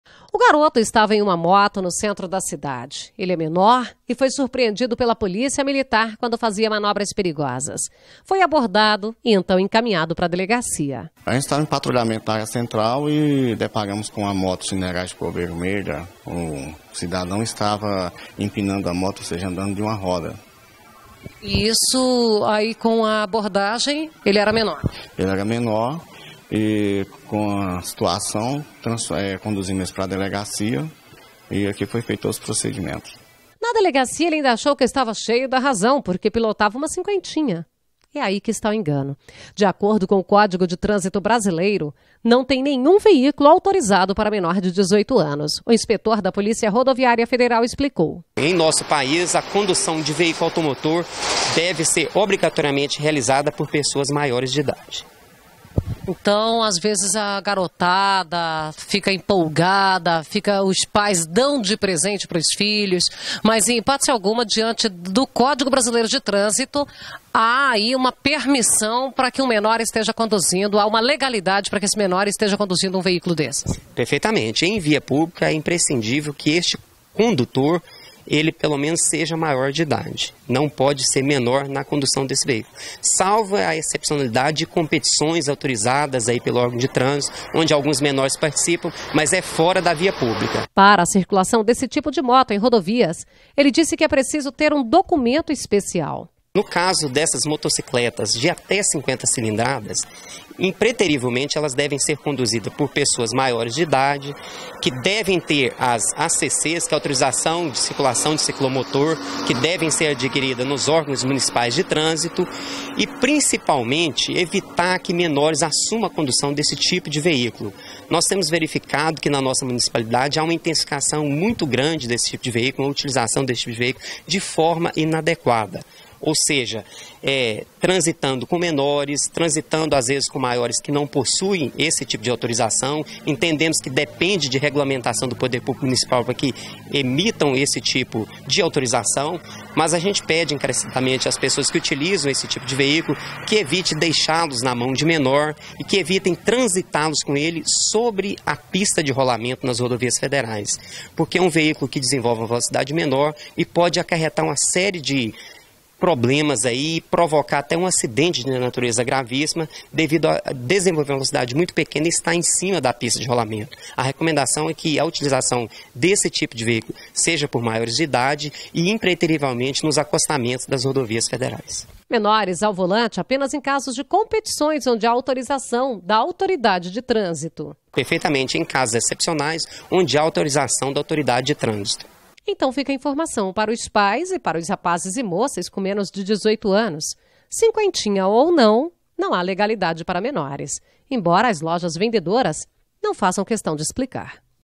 Menores e ciclomotores
Menores e ciclomotores, uma combinação comum, mas é ilegal. Você acha que um menor pode pilotar uma cinquentinha?